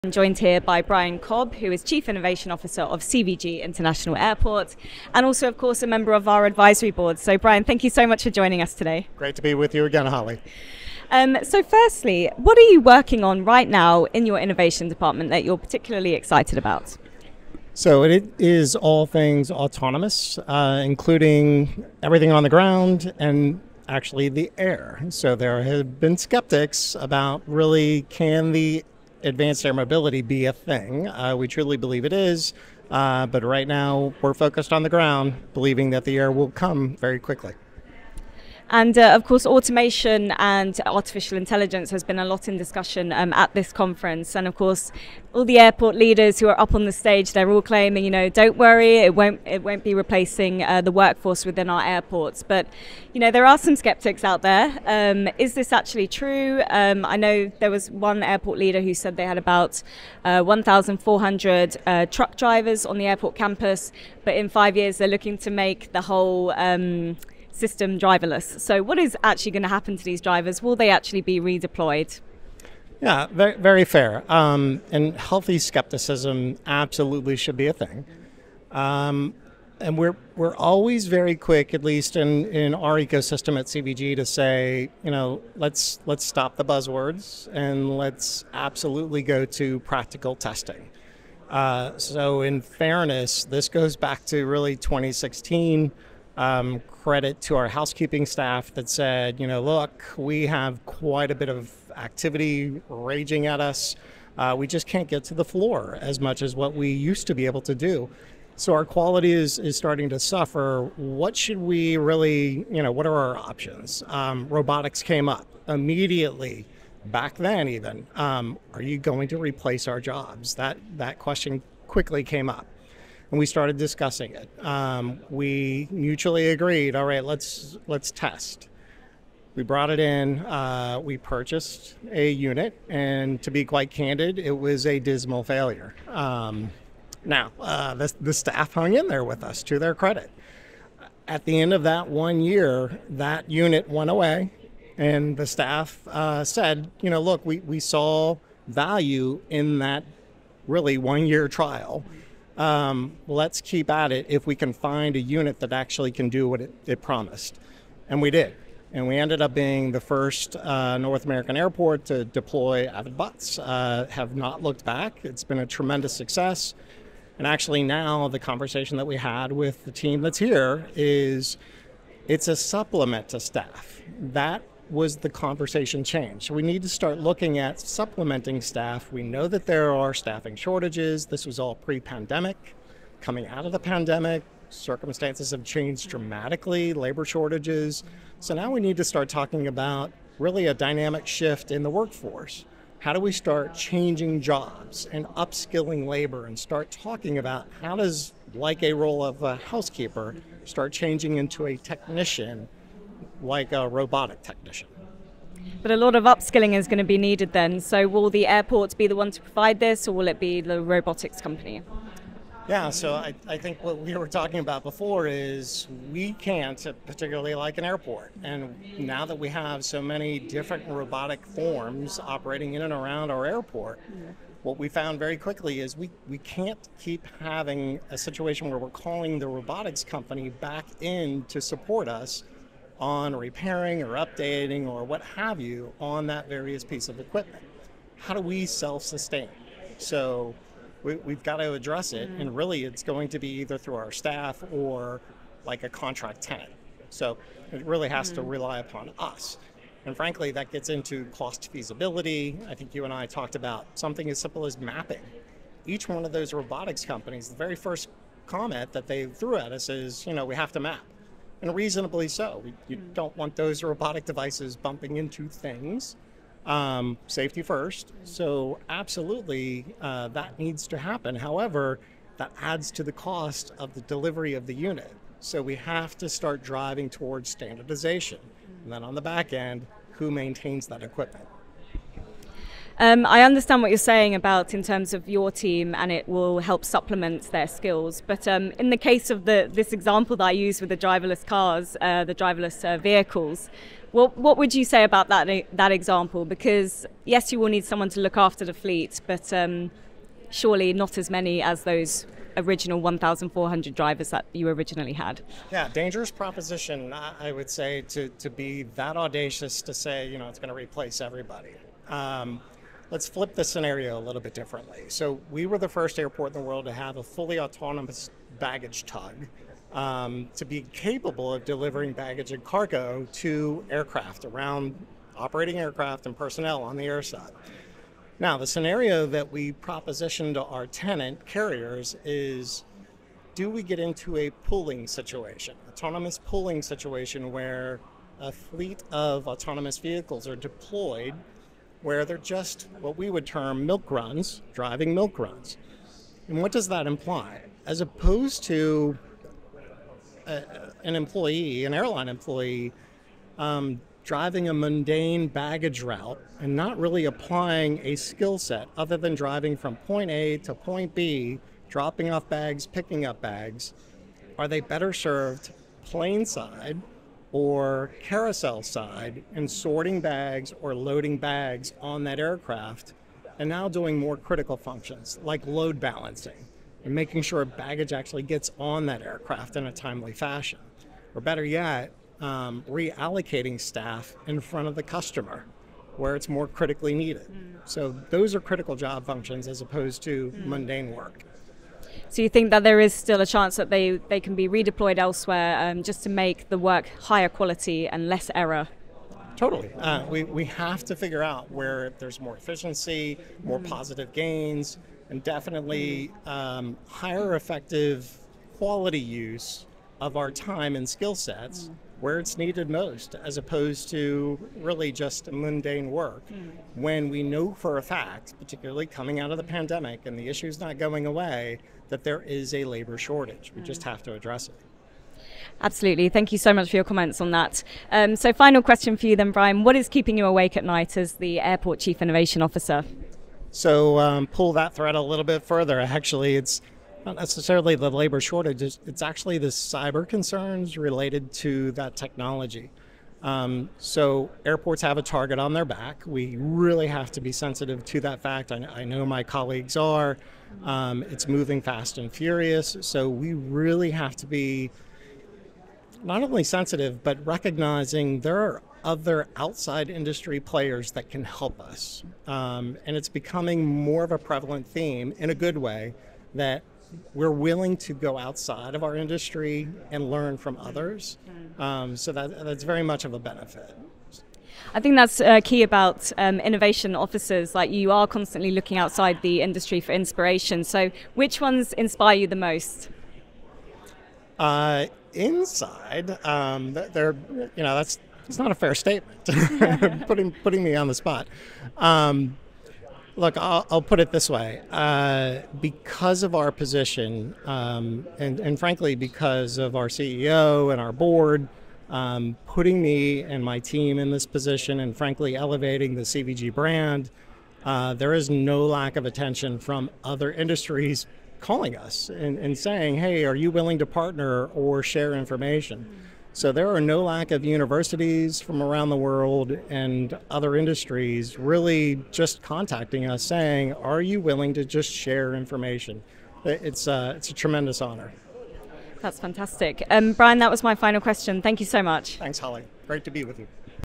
Challenging healthy scepticism around autonomous technology and robotics with Brian Cobb
"How does the role of a house keeper start changing into a robotic technician?"

Brian Cobb, Chief Innovation Officer of Cincinnati / Northern Kentucky International Airport (CVG) discusses the scepticisms around autonomous technology, advanced air mobility and robotics with Holly Miles, Editor of International Airport Review.
Together they unpicked the arguments around whether new technologies will not replace the need for humans in airports, which Brian says is supplementing staff, not replacing.
Brian went on to say that due to the workforce shortage, there is a dynamic shift in the workforce and jobs are changing and there is a need to upskill labour.
Airports must become self-sustaining in being able to repair robotics and train employees. The argument around whether technology will replace jobs is the difference between critical job functions as opposed to mundane work where skills are not used.
What is keeping Brian awake at night is cyber concerns related to this new technology: "Airports have a target on their back," he says.

Watch this exclusive interview with this innovation thought leader now!